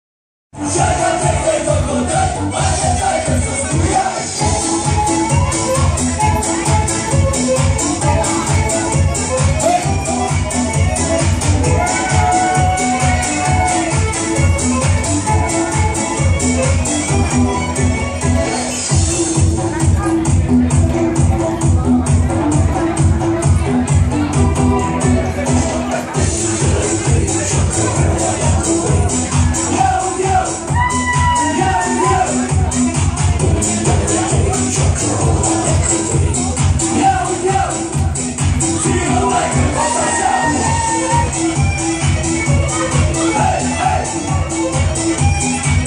Botez Victoria Adriana Mihaela
După un botez tradițional la biserica românească din Toronto, e timpul sa dansam cum știu romanii la o petrecere